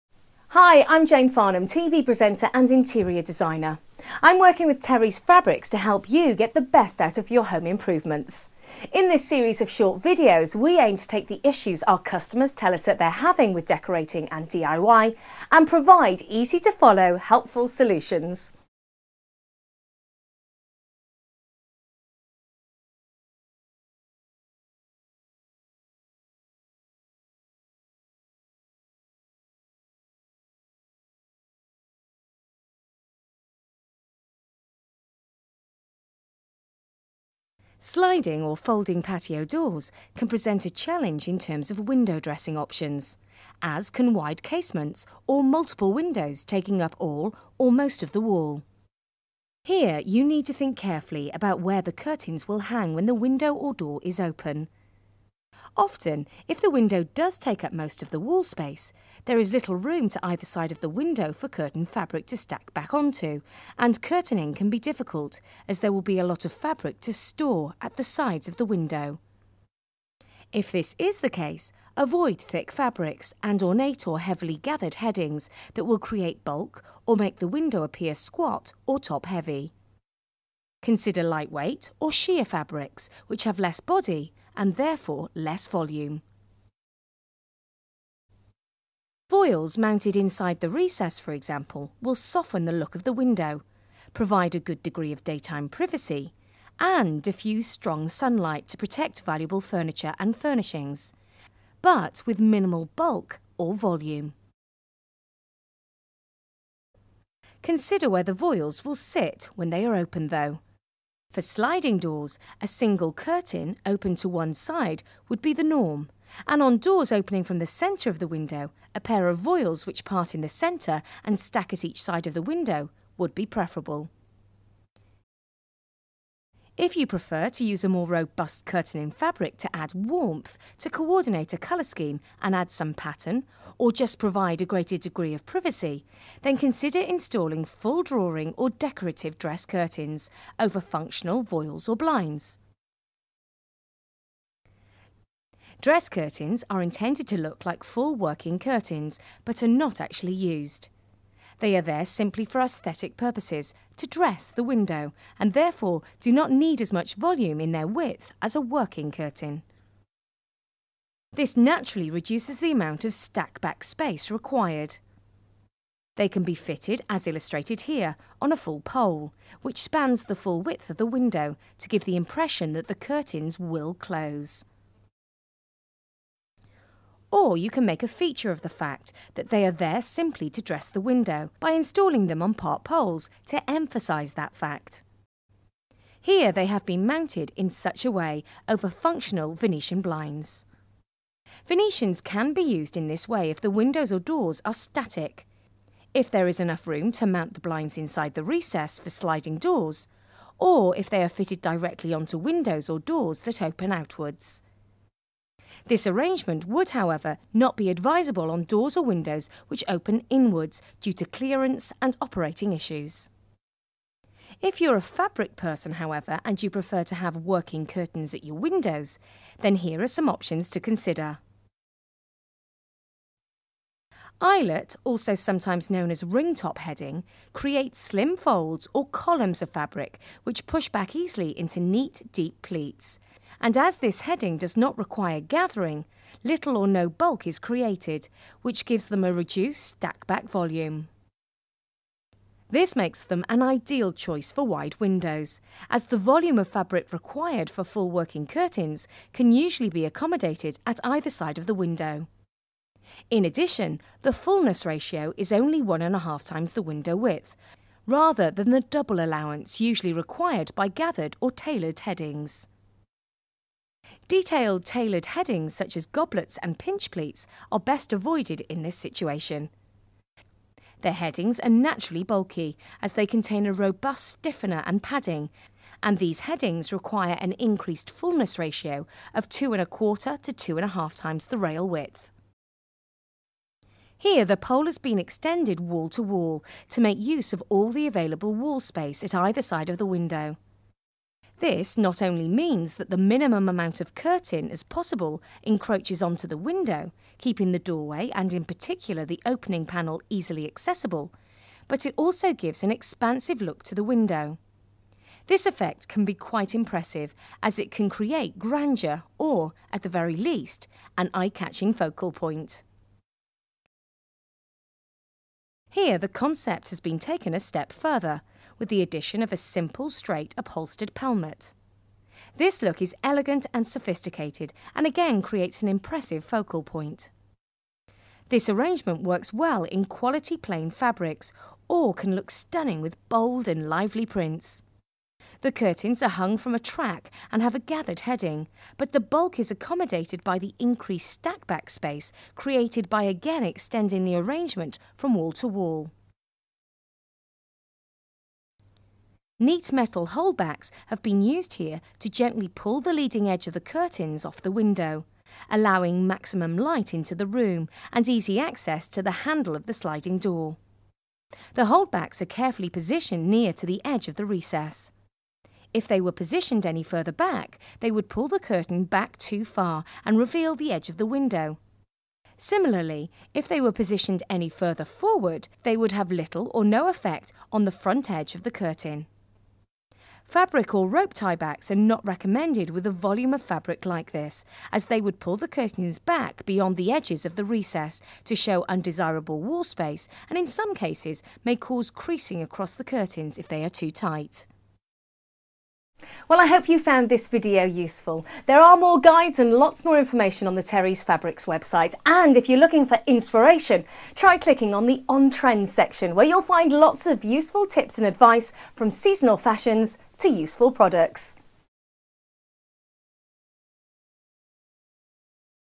How to Dress Windows - for Sliding Patio Doors or Extra Wide Windows
Need some inspiration for how-to dress large windows to achieve a pleasing cohesive decor?
Do you know when to choose thick fabrics or a voile curtain? Jane Farnham answers these questions and presents helpful decoration ideas to make curtains work for extra wide windows.

Post your thoughts and comments below.